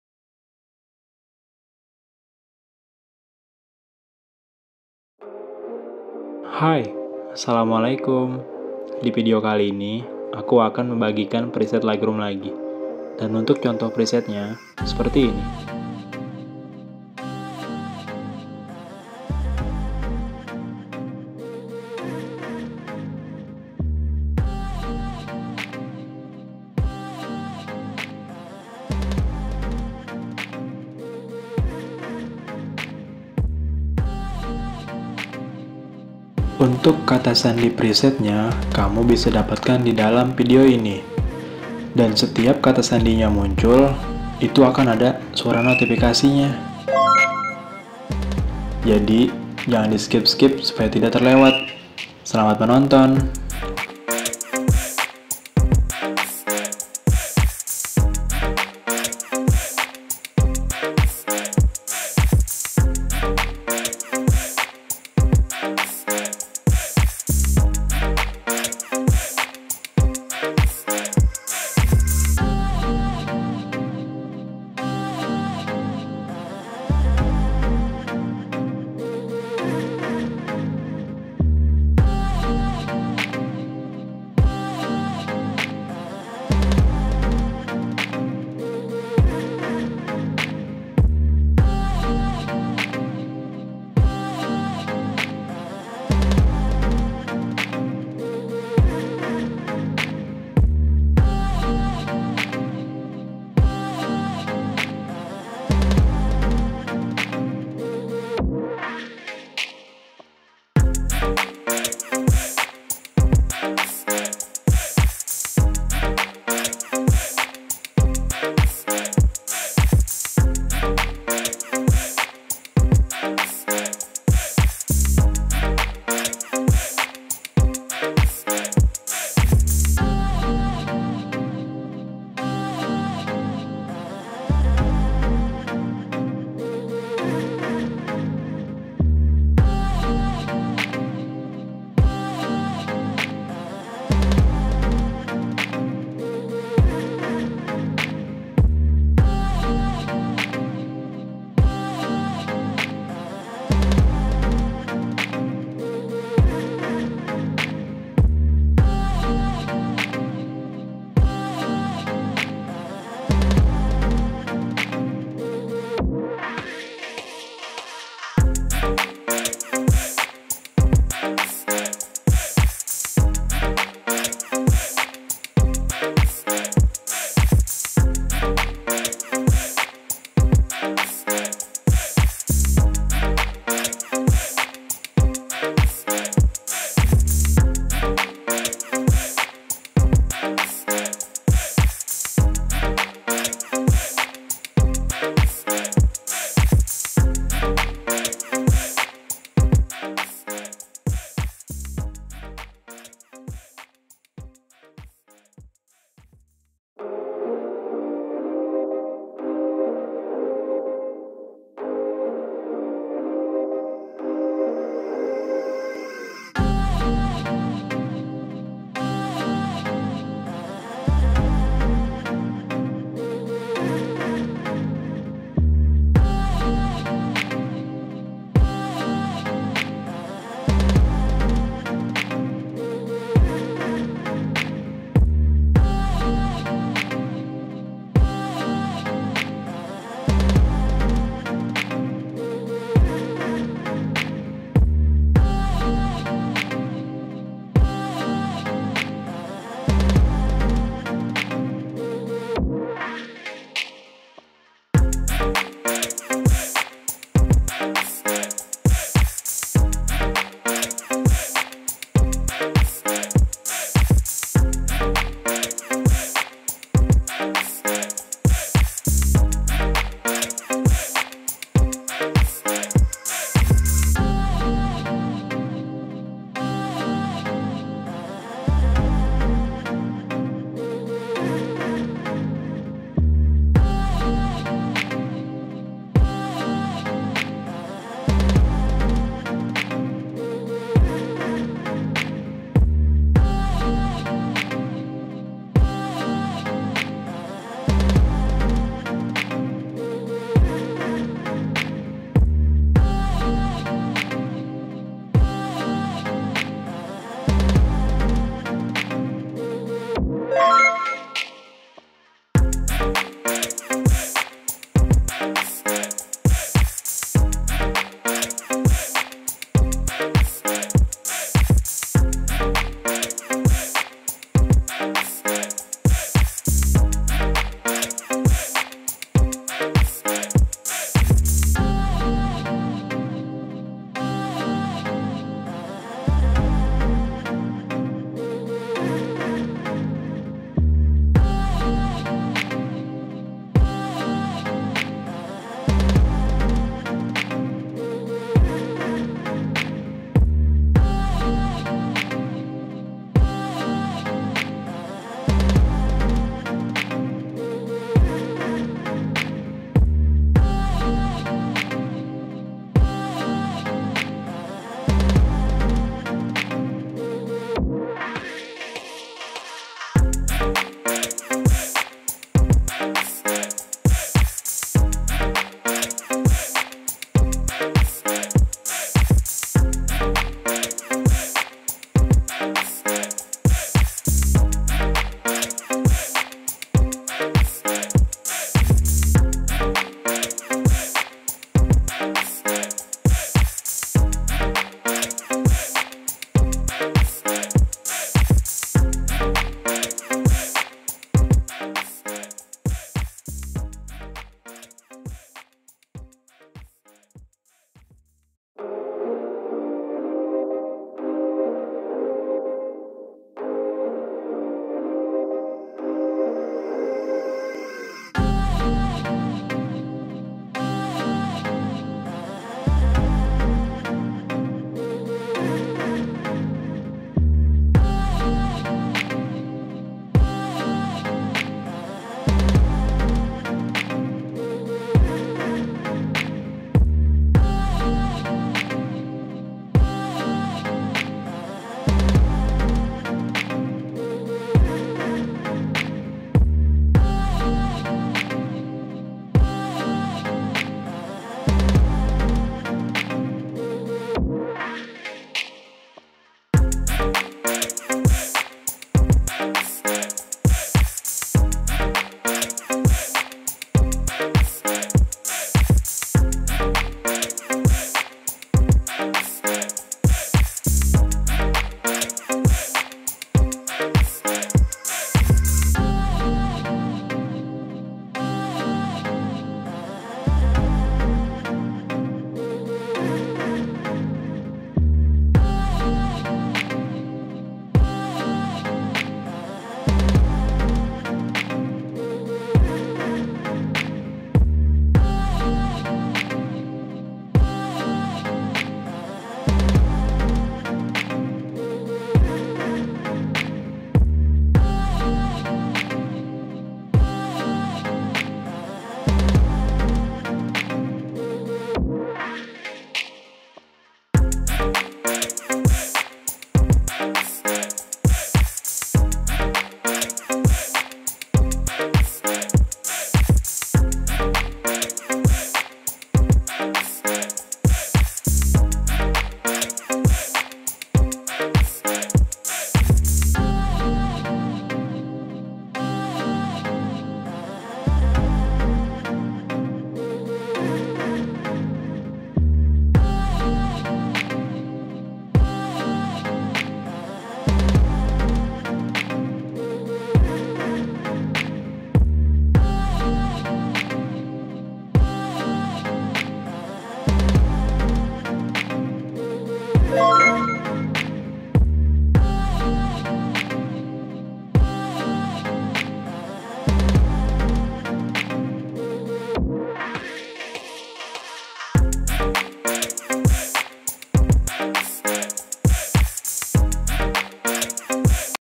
FREE 50+ PRESET LIGHTROOM | TRAVELLER PRESET | LIGHTROOM TUTORIAL
Preset Lightroom yang akan aku bagikan di video kali ini adalah Preset Lightroom Traveller.
Untuk mendapatkan hasil yang maksimal sesuaikan lagi pencahayaan nya dengan keadaan foto kalian ya.

Cara Mendapatkan Password :
📌 Password akan muncul di bagian "KIRI "  video
📌 Cukup satu kali nonton dari awal sampai akhir jangan di skip, Karena nanti akan ada suara notifikasi ketika password muncul
📌 Password terdiri dari 4 digit angka, dan akan muncul secara terpisah (dipisahkan menjadi dua bagian)

Sebagai contoh:
Dalam contoh di atas, password nya adalah 1234.

Preset Untuk Pengguna Android
🔻Preset XMP (MediaFire) :

Preset Untuk Pengguna iPhone
🔻Preset DNG (MediaFire) :

Gunakan Google Chrome untuk mengunduh Preset.

Gambar : Instagram
Semua gambar yang ditampilkan dalam video adalah milik pemiliknya masing-masing. Semua gambar hanya untuk tujuan ilustrasi.